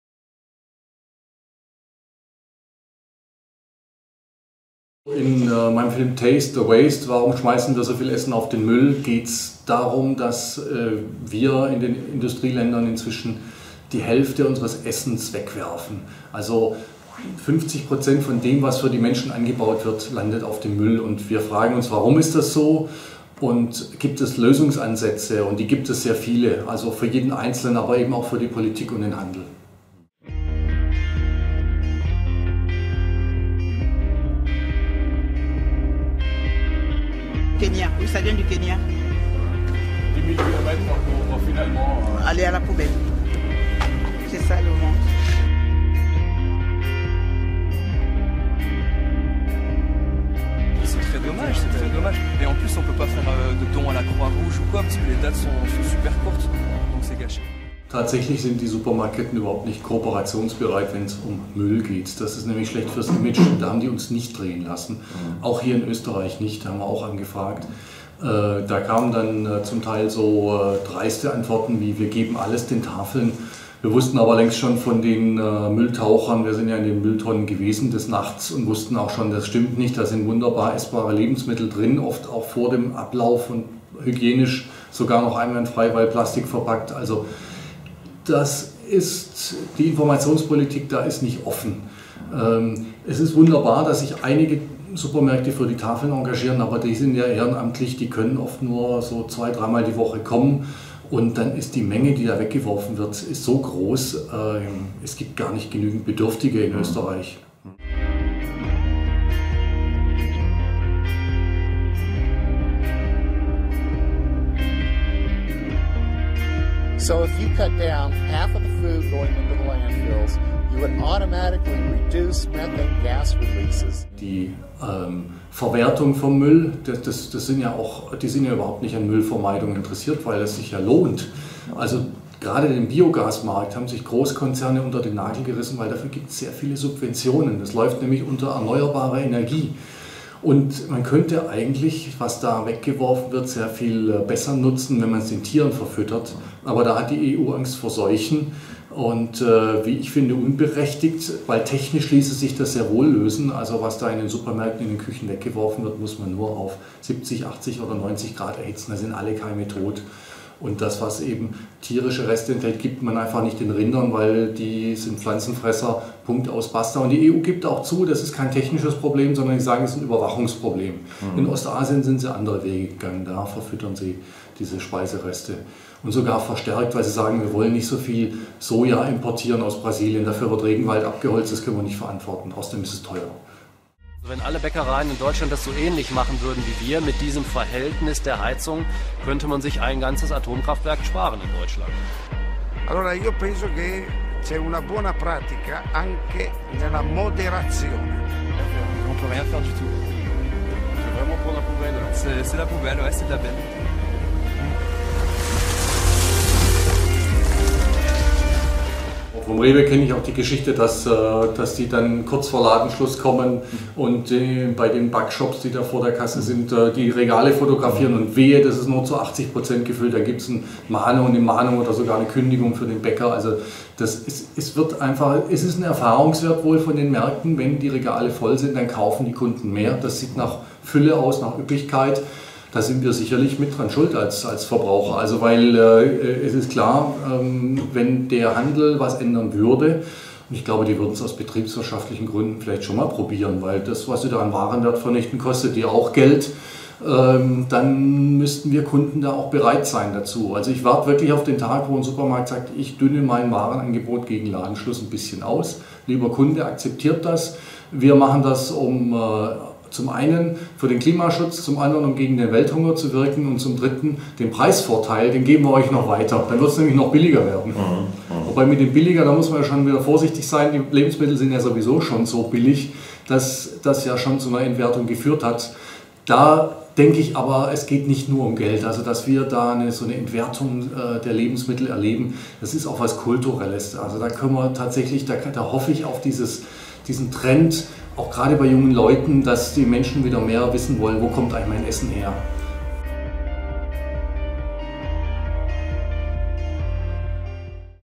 VALENTIN THURN über seine Doku TASTE THE WASTE // Interview
In diesem kurzen Interview spricht der deutsche Regisseur Valentin Thurn über seinen Dokumentarfilm "Taste the Waste", die Wegwerfgesellschaft und warum die Konsumenten auch mithelfen können, wenn es darum geht, zu verhindern, dass Nahrungsmittel auf dem Müll landen. Aufgezeichnet von Matthias Greuling, Oktober 2011. Mehr Infos zu unserem Filmmagazin